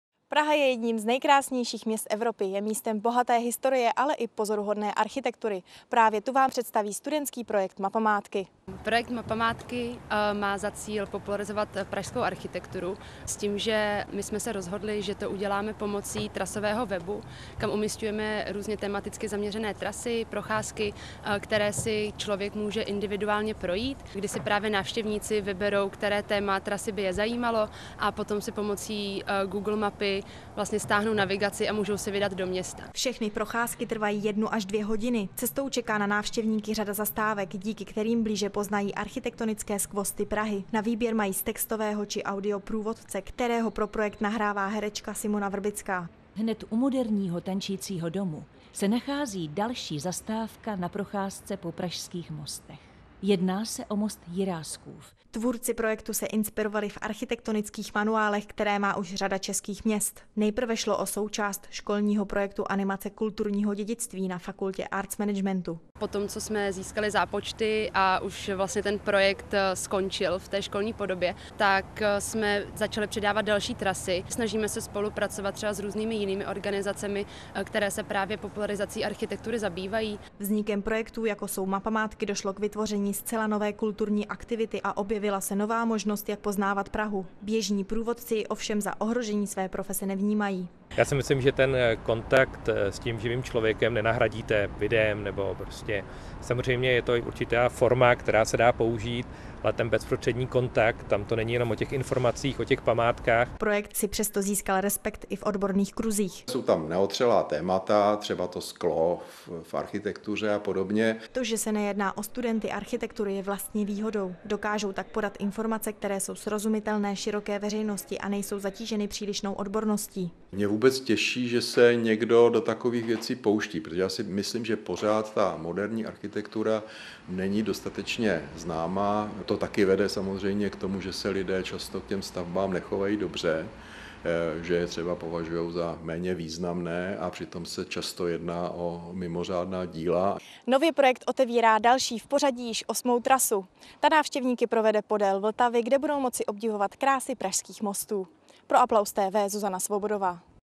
Projekt Mapamátky popularizuje pražskou architekturu
Tvůrci projektu Mapamátky vytvořili platformu, která vás zavede do pražských ulic. Stačí si vybrat z tématicky zaměřených tras, spustit navigaci a můžete začít poznávat pražskou architekturu. Jednotlivé zastávky vám například přiblíží moderní architektonické styly jako jsou funkcionalismus a český brutalismus nebo se můžete vydat po stopách architekta Karla Pragera. Volba je jen na vás a vyrazit můžete kdykoli se vám zachce!